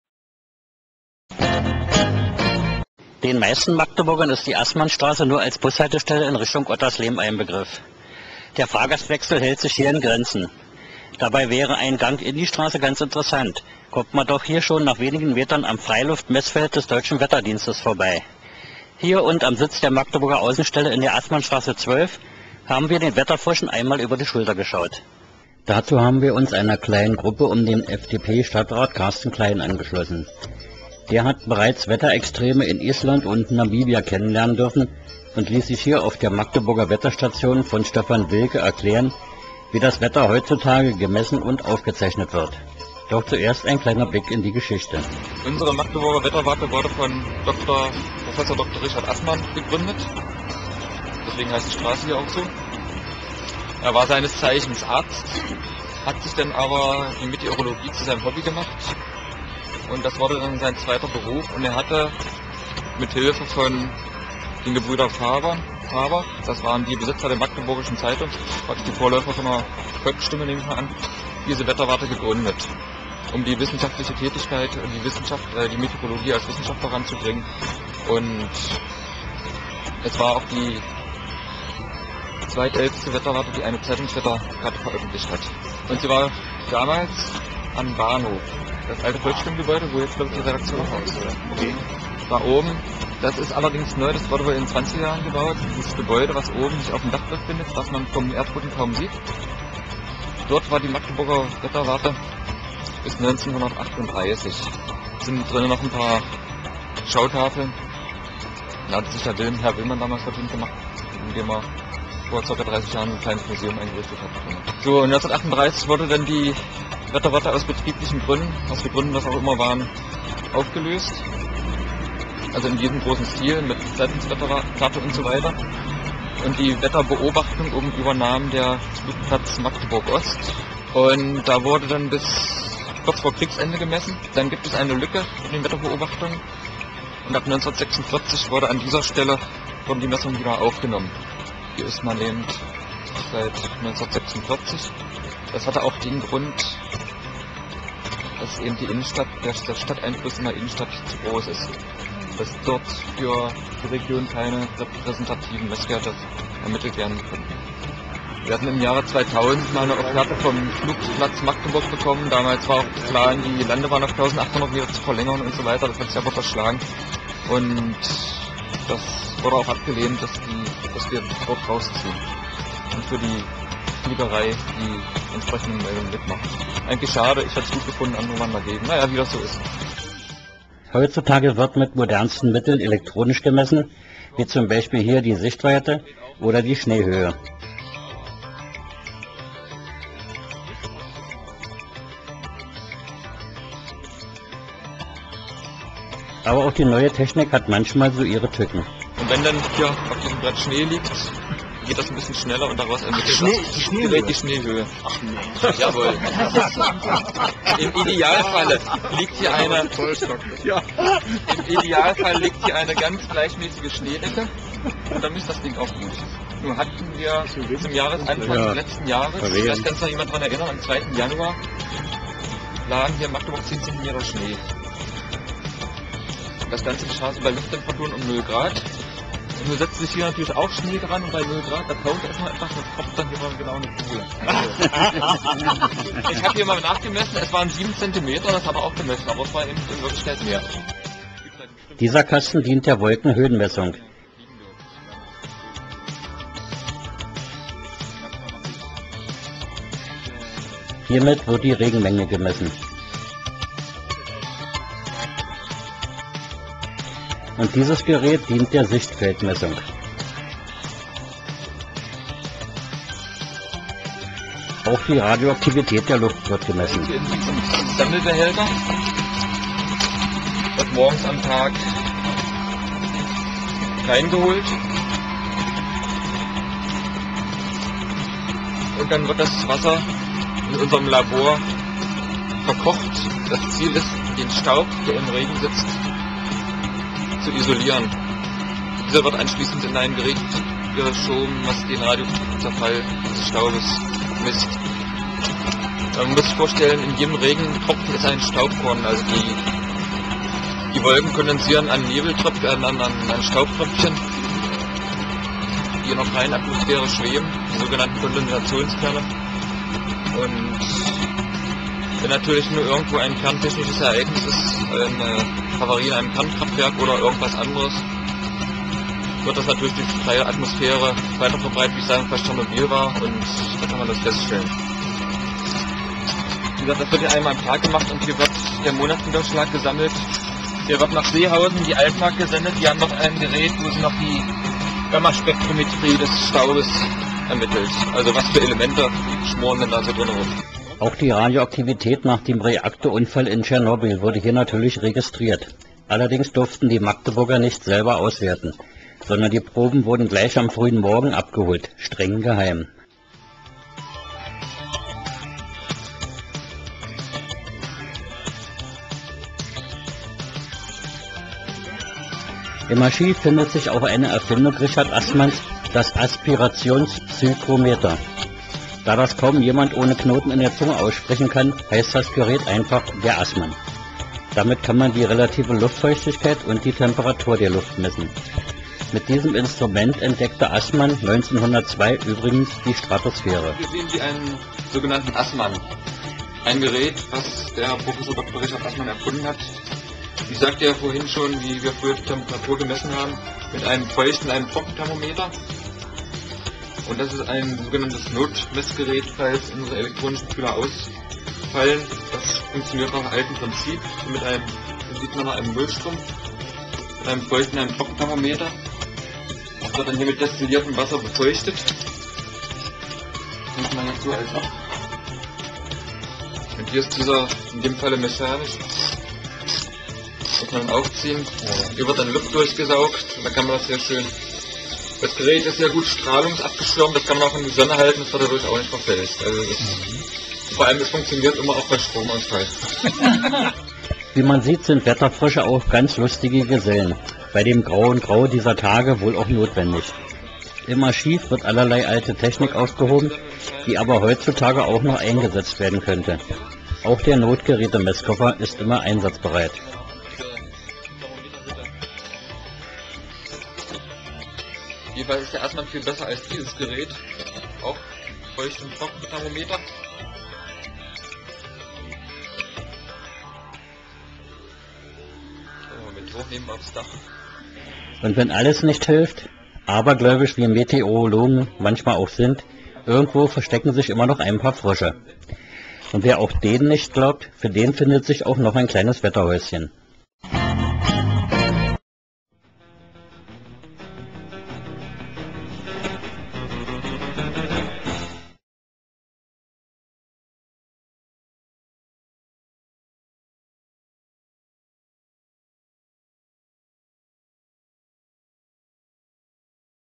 Besuch bei den Magdeburger Wetterfröschen
Den meisten Magdeburgern ist die Aßmannstraße nur als Bushaltestelle in Richtung Ottersleben ein Begriff. Der Fahrgastwechsel hält sich hier in Grenzen. Dabei wäre ein Gang in die Straße ganz interessant, kommt man doch hier schon nach wenigen Metern am Freiluftmeßfeld des Deutschen Wetterdienstes vorbei. Hier und am Sitz der Magdeburger Außenstelle in der Aßmannstraße 12 haben wir den Wetterfröschen einmal über die Schulter geschaut.